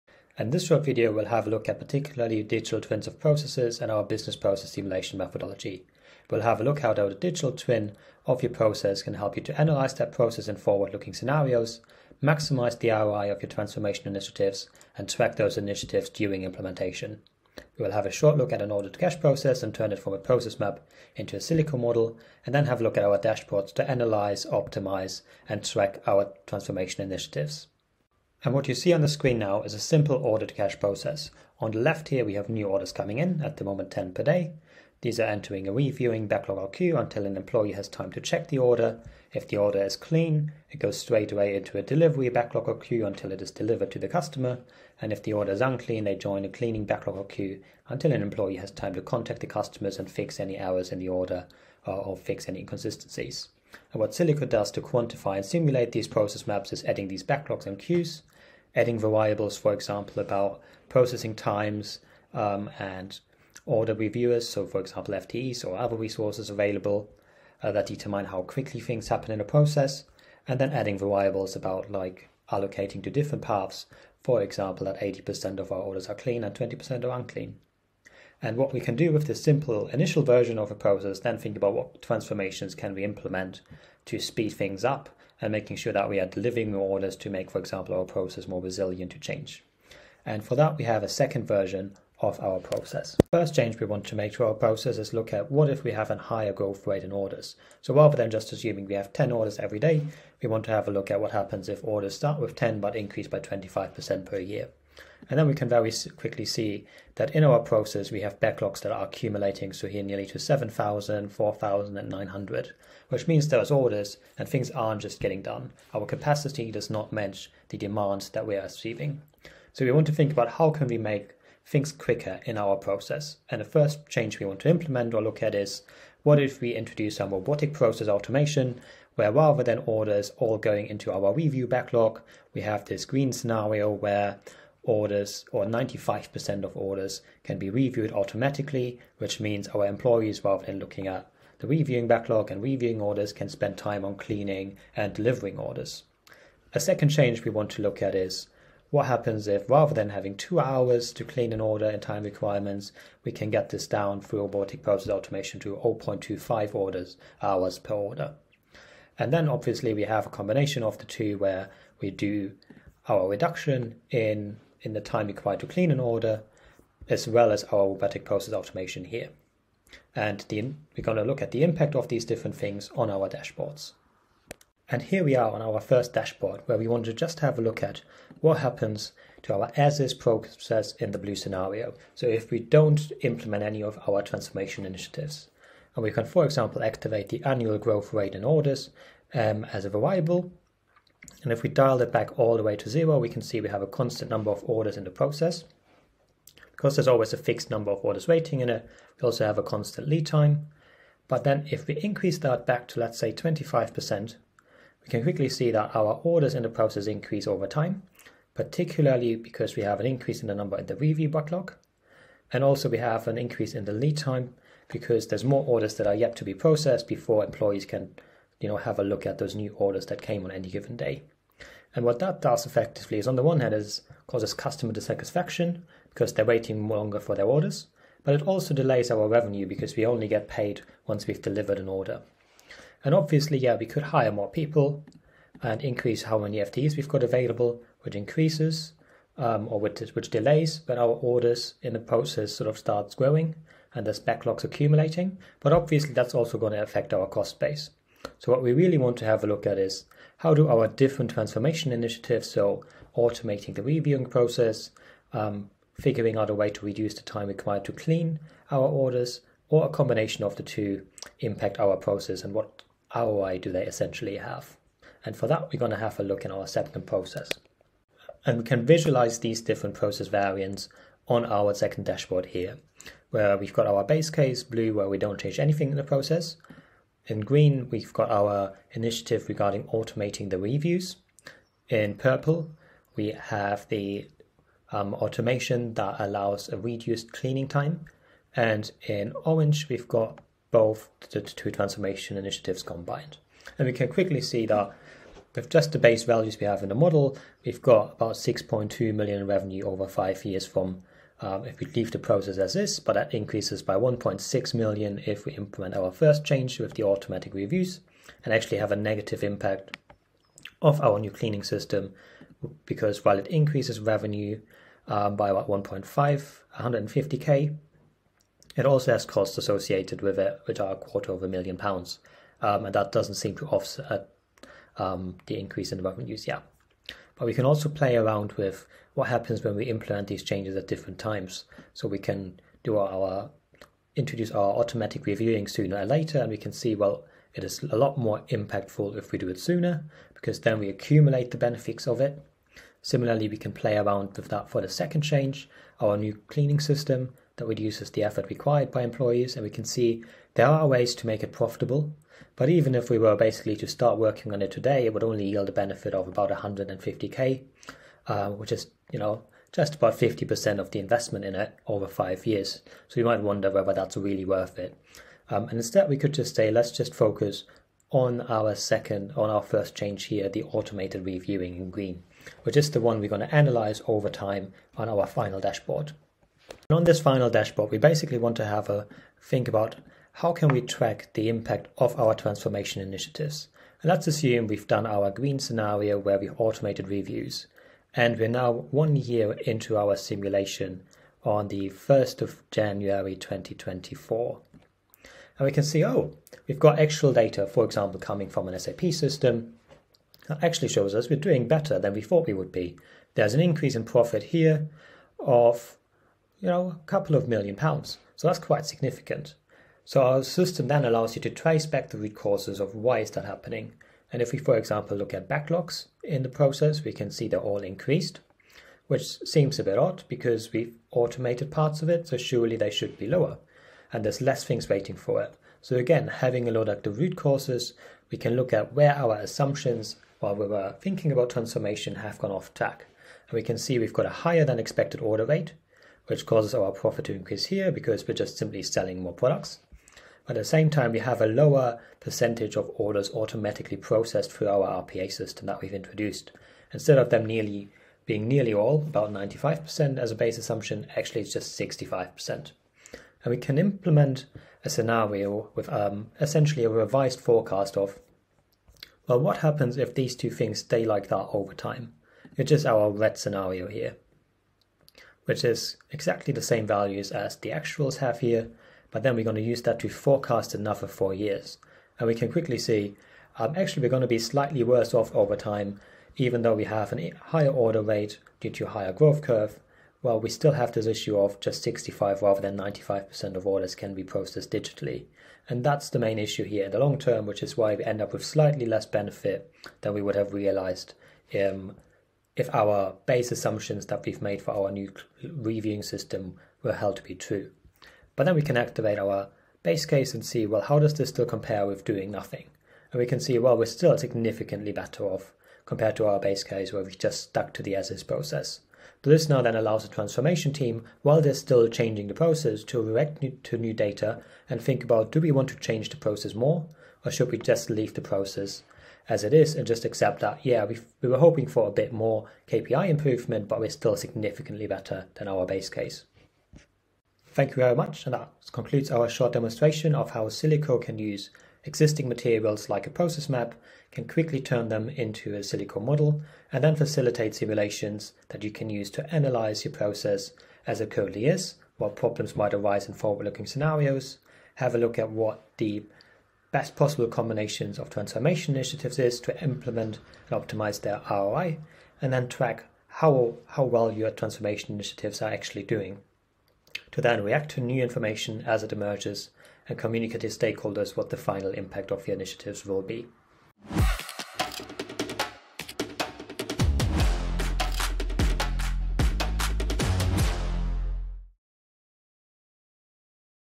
How to Analyse the Transformation Initiatives of an Order-to-cash Process
This video takes you through the creation of digital twins of processes and the Silico Business Process Simulation (BPS) methodology. We will also cover how a digital twin of a process can help you analyse the process in forward-looking scenarios, maximise the ROI of your transformation initiatives and track them during implementation.

Learn how Silico dashboards can help you track, analyse and optimise your transformation process improvement strategies.

00:28 - Introduction to an Order-to-cash process simulation model
01:32 - What if we have a higher growth rate in orders and capacity doesn’t meet demand?
01:58 - How can we improve the Order-to-cash process with RPA?
02:26 - What happens if we can reduce order cleaning time from 2 hours to 0.5 hours with RPA?
02:38 - What happens if we use a combination of RPA methods to improve the order-to-cash process?
02:54 - How do the Order-to-cash transformation initiatives compare on the Silico dashboard?
03:53 - What happens to the Order-to-cash process if you increase the number of FTE’s?
06:17 - How can we track our transformation initiatives over with RPA changes 1 year in?

If you'd like to read more about transforming order-to-cash and lead-to-cash processes within a customer case study,